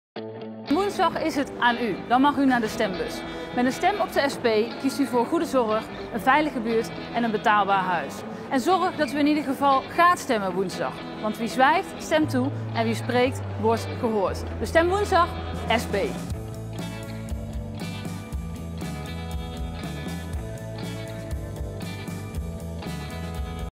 Lilian flitssocialist in Nijmegen
Lilian Marijnissen is AltijdindeBuurt. Zo flitste ze door Nijmegen met een stemadvies: Kies voor elkaar, stem ook in Nijmegen SP!